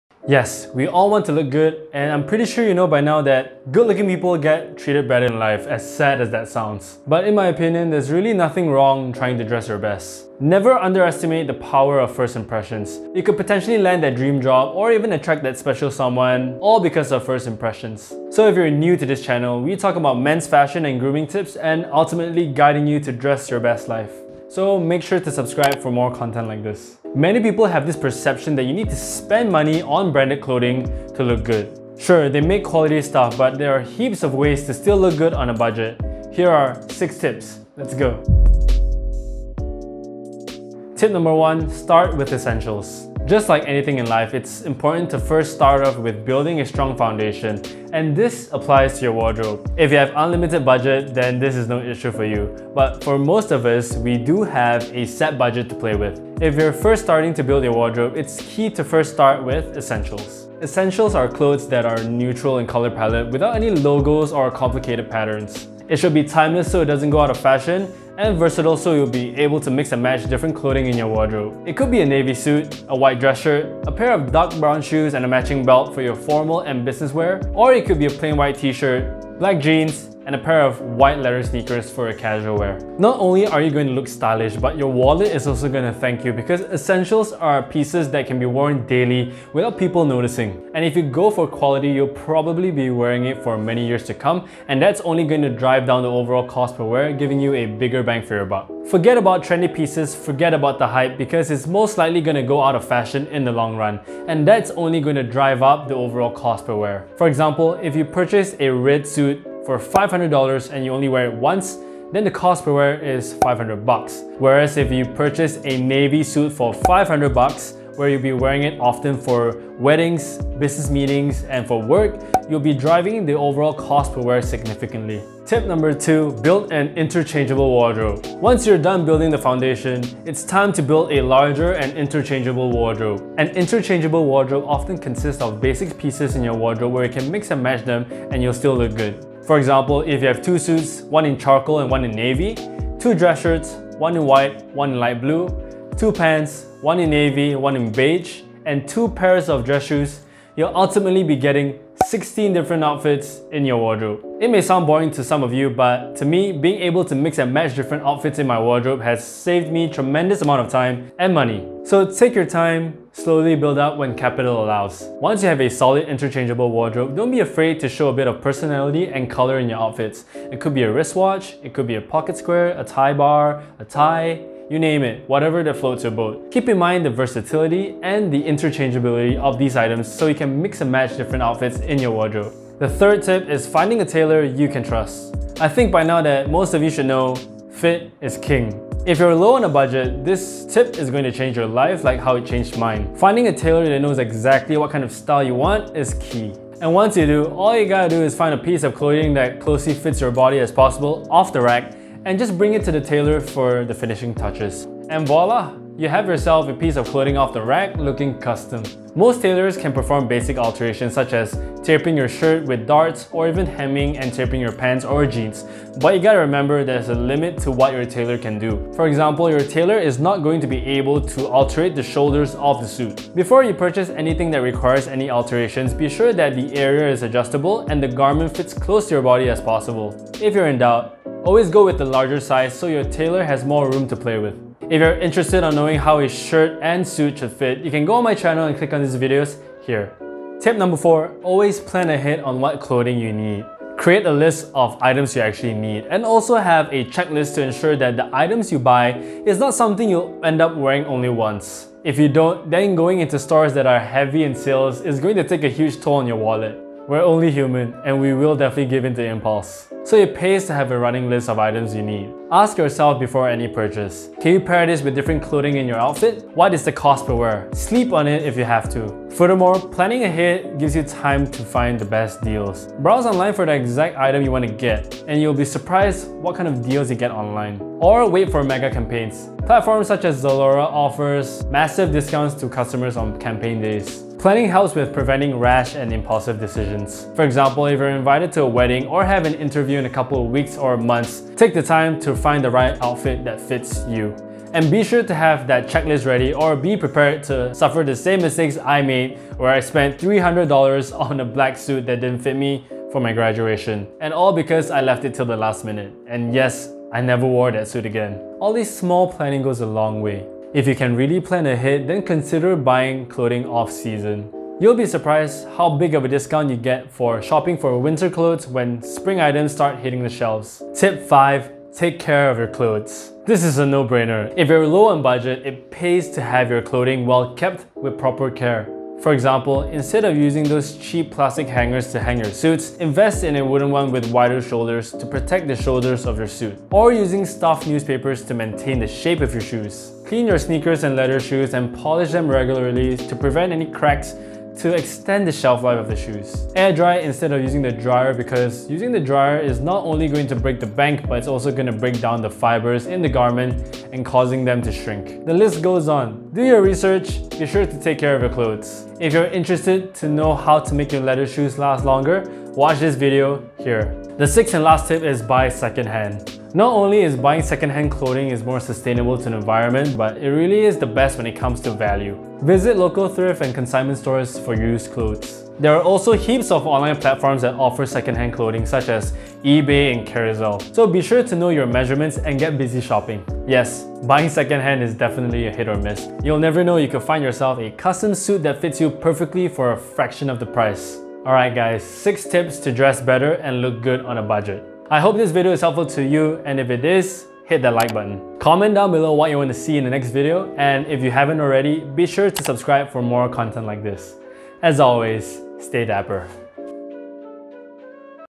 How To Look Good On A Budget | 6 TIPS TO LOOK GOOD ON A LOW BUDGET
Not much in the bank but still want to look good and dapper? In this video, we talk about how to look good on a budget.

There is always a reason, you want to look good. Job interview, stunning date to impress. Whatever the reason, looking and dressing well doesn't mean you need to buy branded clothing that is worth over hundreds or thousands of bucks and having the Supreme or Louis Vuitton logo across your chest doesn't make you more stylish at all.

Don't get me wrong, I definitely appreciate certain brands and they definitely produce high quality clothing but if you're just starting out with building your career and don't have enough to afford it then it will be wiser to invest in the classics and timeless pieces for your wardrobe first.

Stylish men create their own style and not just purchase trendy pieces that might just work for ONE season and go out of fashion in the next. Focusing on practicality above all else will save you a lot in the long run. Here are 6 tips to look well dressed and good on a budget.

Timestamps:
00:00 - Intro
00:40 - Start With Essentials
02:05 - Build An Interchangeable Wardrobe
03:02 - Find A Tailor You Can Trust
04:00 - Plan Ahead On What Clothing You Need
05:20 - Take Care of Your Clothes
06:04 - Buy Second Hand
06:31 - Outro